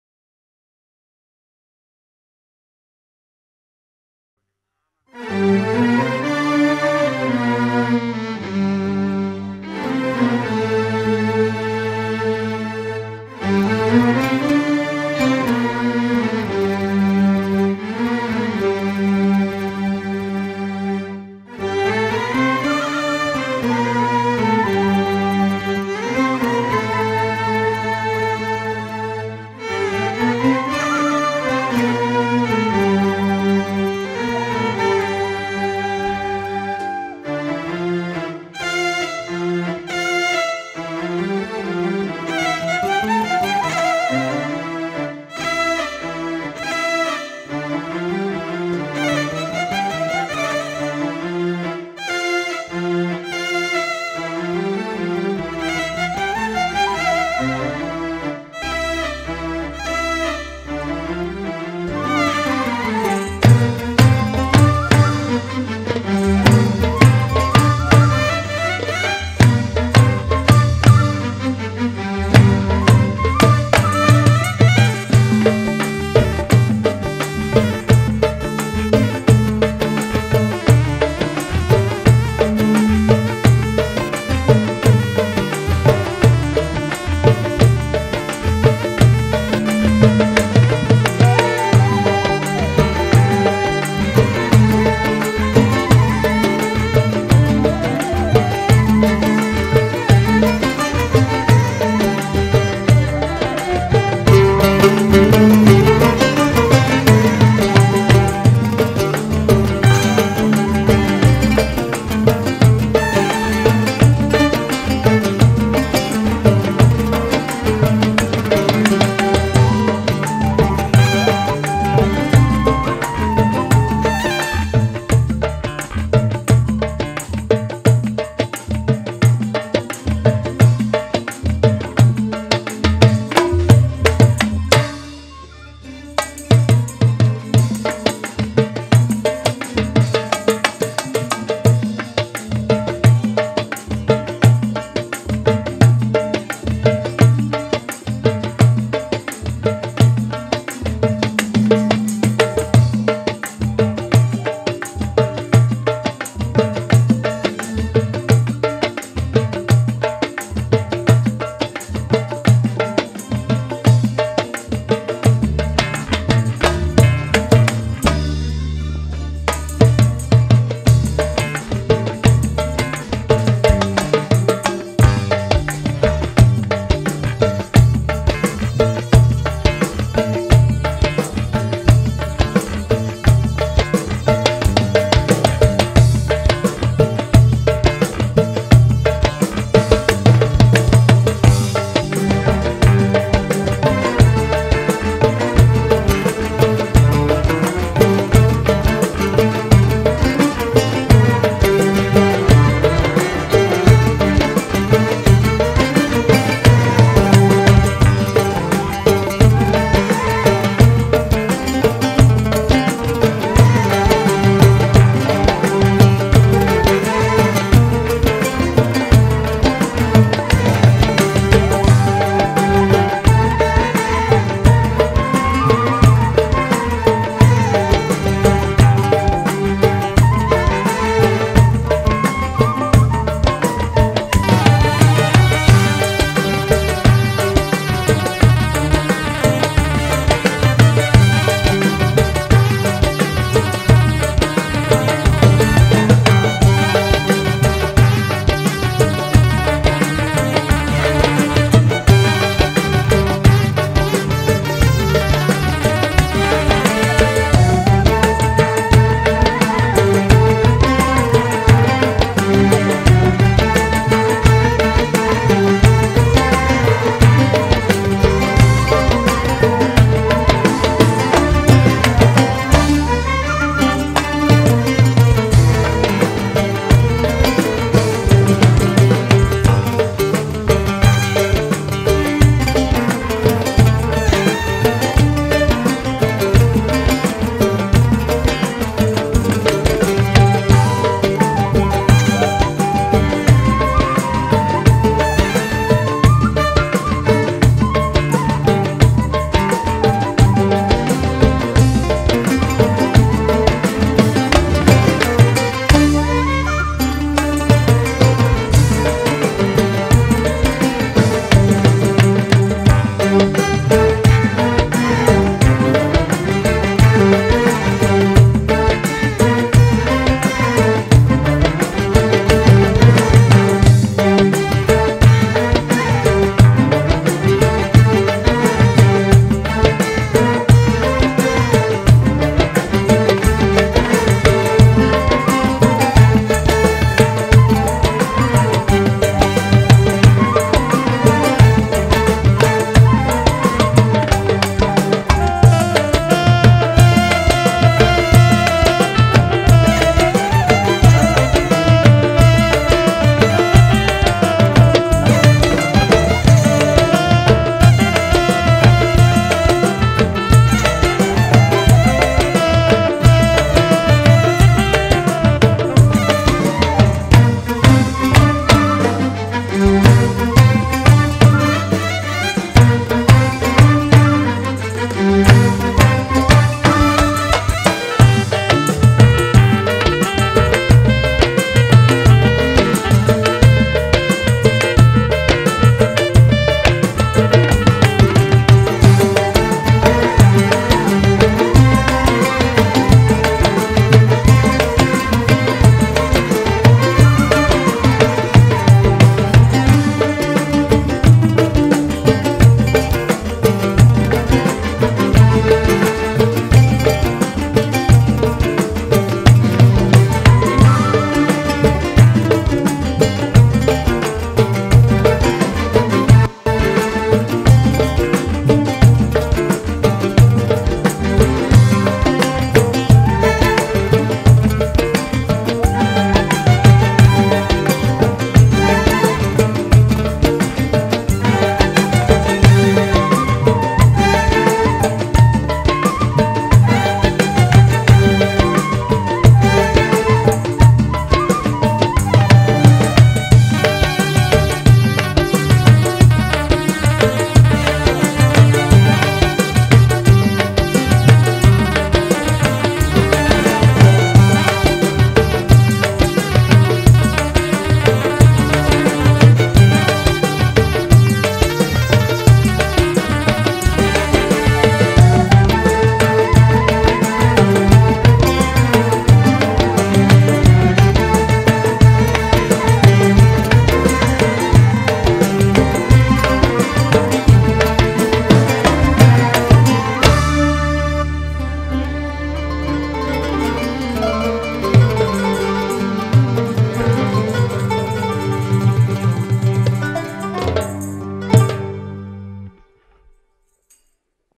INSTRUMENTALIA AL IFRAH 2021
LIVE AL IFRAH DI DESA RAGUNG PANGARENGAN SAMPANG
INSTRUMENT LAGU TANPA KATA  AL IFRAH 2021